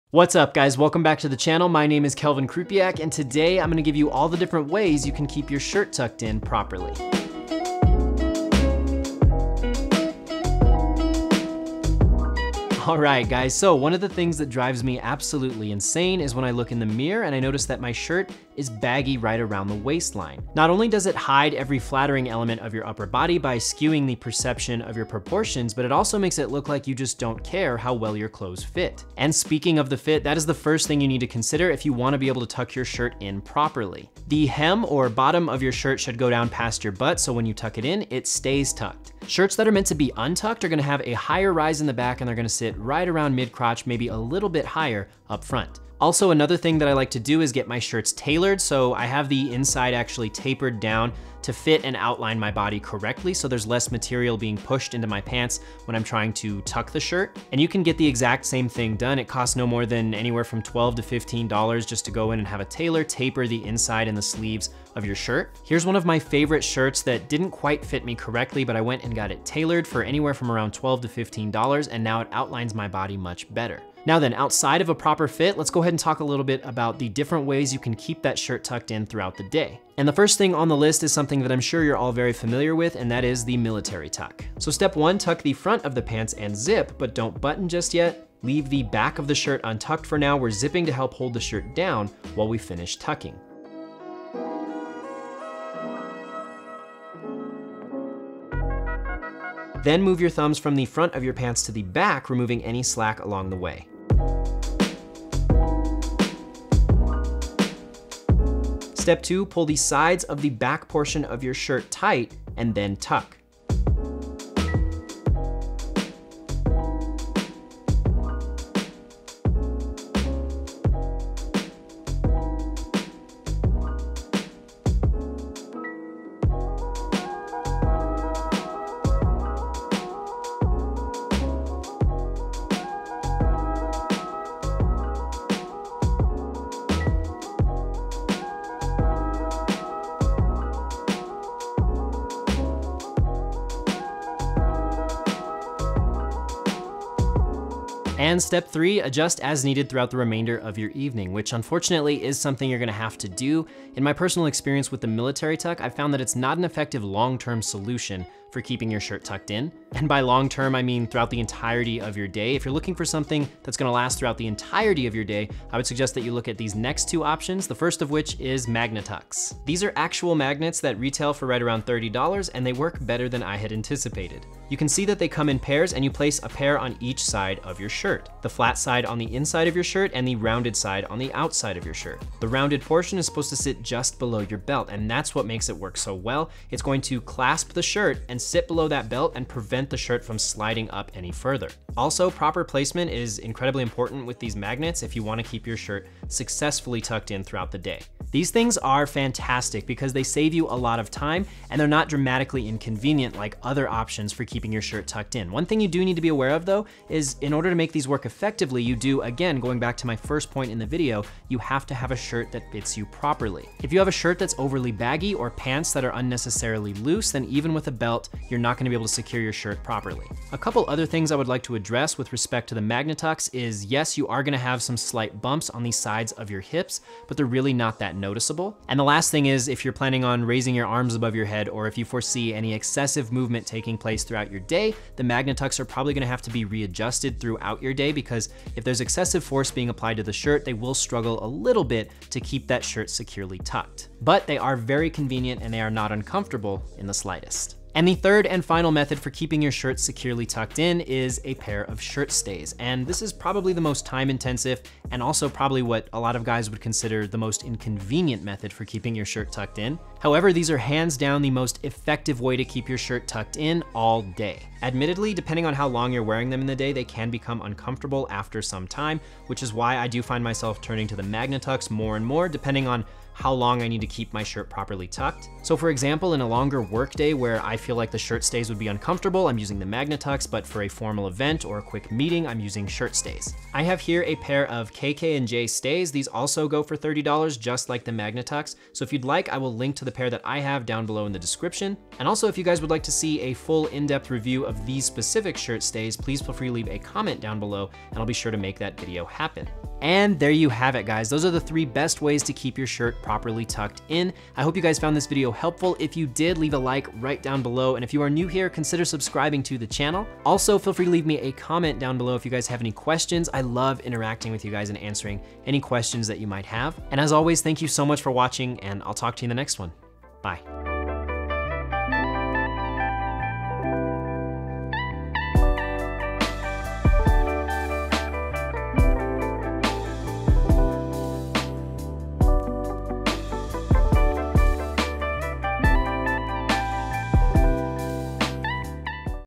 Best Way to Keep Your Shirt Tucked in (ALL DAY)
What's up guys today I'm showing you what I would consider the best 3 methods for keeping your shirt tucked in throughout the day.

#1 - The Military Tuck

I'm sure many of you are familiar with this one. I myself learned about it at an early age and executed it wrong for years. There were little details in this style of shirt tuck that I just wasn't figuring out. I would consider this an effective short-term tucking solution. I say short-term because You'll almost never make it through the whole day with a military tuck. You'll find yourself readjusting it again and again as the day progresses.

This is a really great idea that (IMO) works really well. I was skeptical of how well these could actually hold your shirt down but after using them for months I'm 100% convinced of their effectiveness. These come in pairs and the magnets clamp onto your shirt and use the belt to prevent the shirt from sliding up any further. So obviously you'll need a belt for these to work but I imagine in most situations where you're wearing a dress shirt you'll have a belt.

The one place where these do fall a little short is during any quick moments of "excessive" movement. If you raise your arms above your head, for example, that should be enough force to pull the Magnetucks apart. However, I find that those moments are few and far between and this product makes up for any shortcomings by being the most convenient and comfortable solution on this list.

#3 - Shirt Stays

These are hands down the most effective solution for keeping that shirt tightly tucked all day no questions asked. I'm currently testing out a pair of KK&Jay stays and so far I've had no problems with them. The secure at two points on the shirt and then one point on your socks to lock the shirt into place.

Admittedly they can become somewhat uncomfortable throughout the day. So I've found my self-using a combination of these and the magnetucks more and more depending on how long of a day I'm planning on having. For example, if I have a long work day ahead of me I'll probably use magnetucks because they'll be more comfortable for a longer period of time.

If I'm going to a wedding or a meeting then I'll probably use the shirt stays because I won't have to wear them all day.